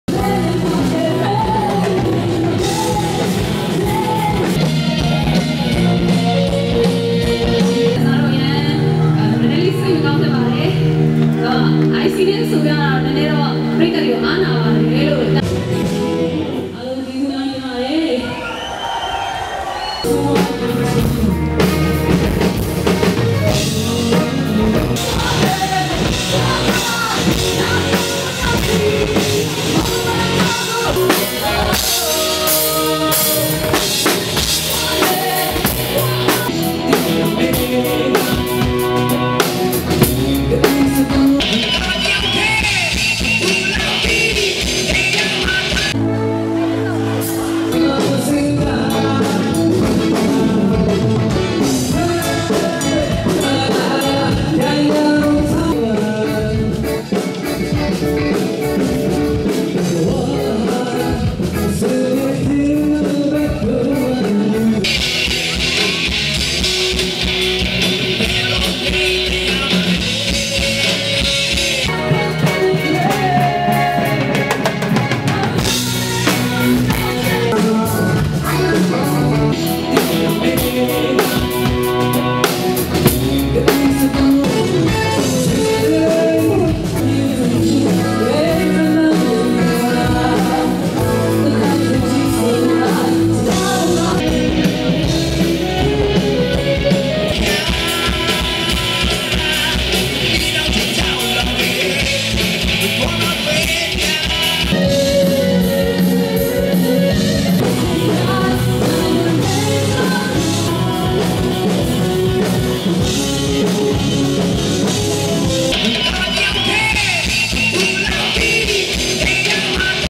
DVB - ေမွ်ာ္စင္ကြ်န္းမွာ အိုင္စီသၾကၤန္ ဘယ္လိုၿကိုုခဲ့ၾကသလဲ
၂၀၁၅ ခုနွစ္ ေမွ်ာ္စင္ကြ်န္း IC Show ရွိဳးၿမင္ကြင္း ရိုက္-ခင္နွင္းၿဖဴစိုး ၊စည္သူေဇယ်